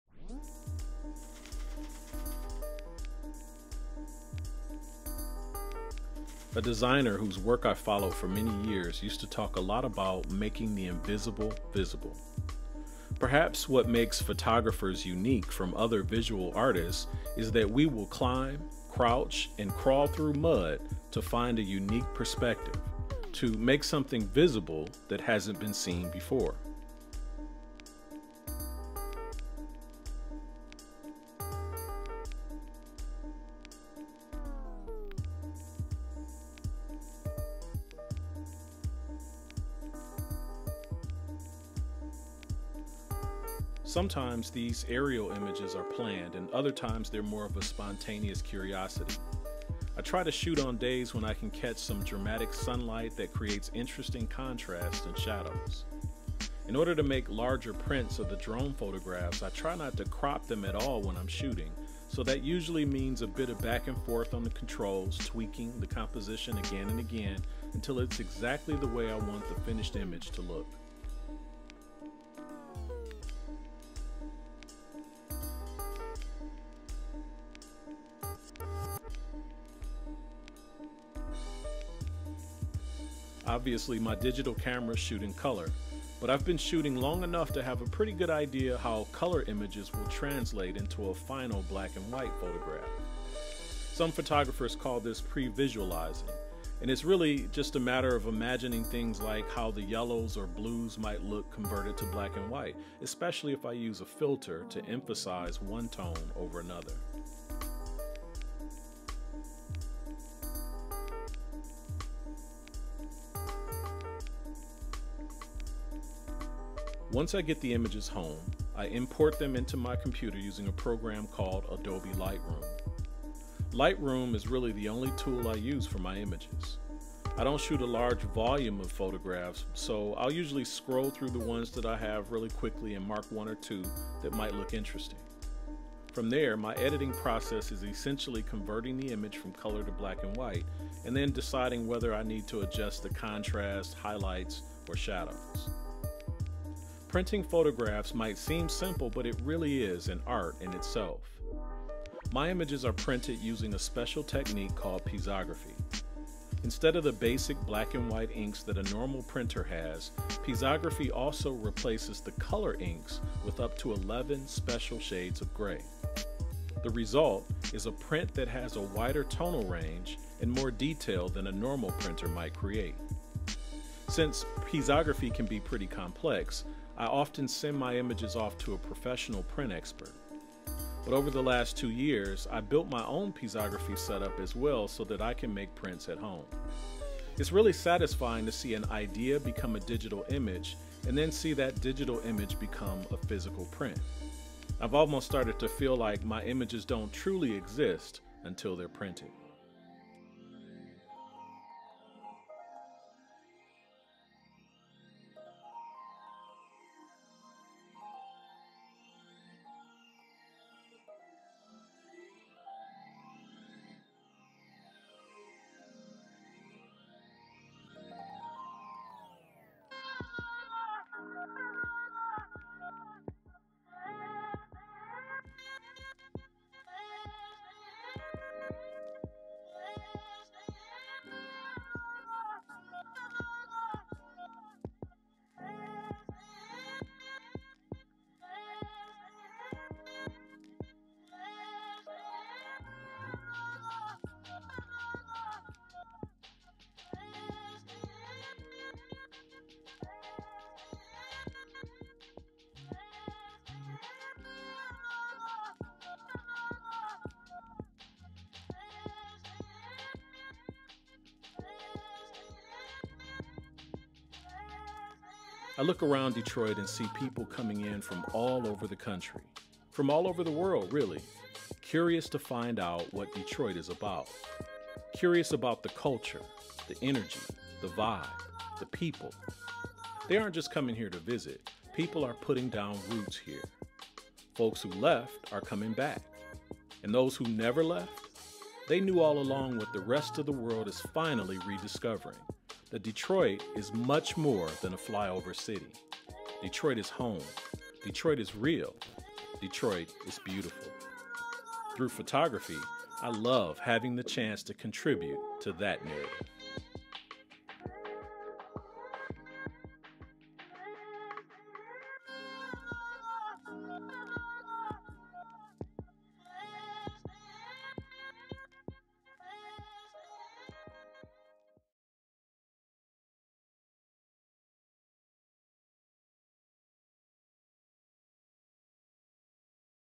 Detroit From Above
Detroit From Above, a short video discussing my aerial drone photography in Detroit.